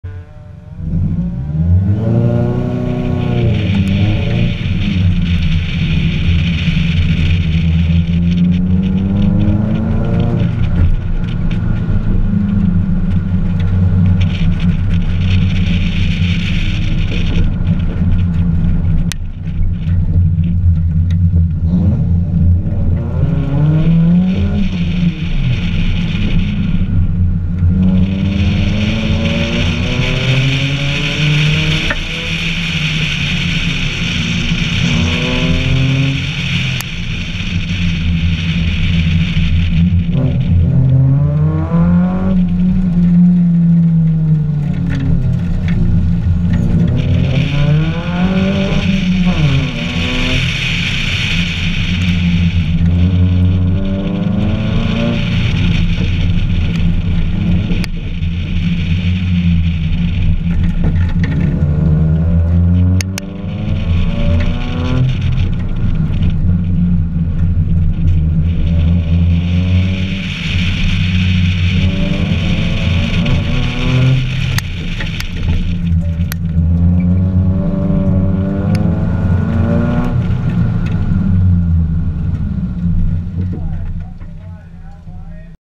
Verona Autocross First Run
My second time participating in an autocross event. I won my class and had a lot of fun. I'm running a Mazda3 N/A 2.0. I was put into DSP which isn't the right class I should have been in FSP or SMF I think. Mods are intake, header, bigger rear sway bar, coilovers, 17x9 Gramlight wheels with a 245 Bridgestone Potenza RE-11A tire.